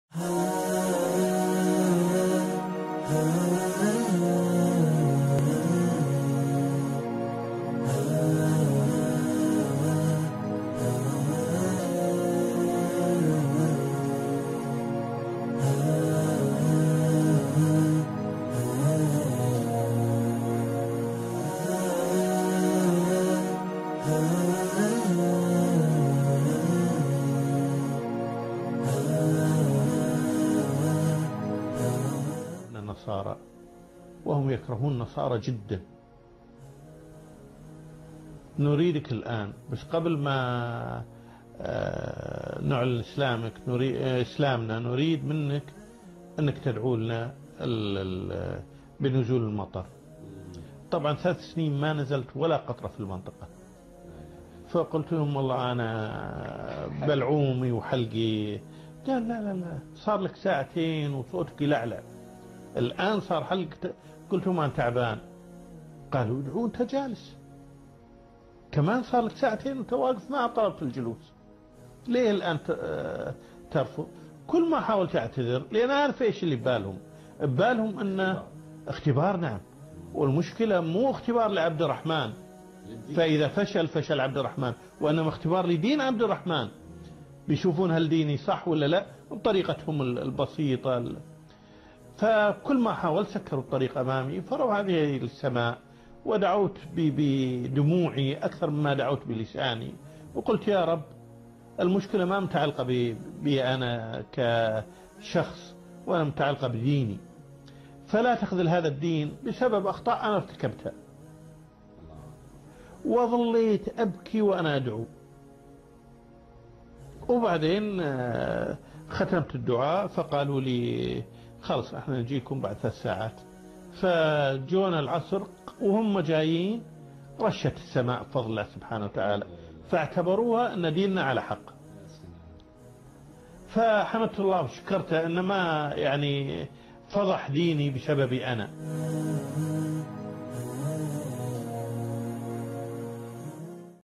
Great Muslim Figures #3 - Abdulrahman Al Sumait
Watch how Abdulrahman Al-Sumait was tested by an African village while calling them to Islam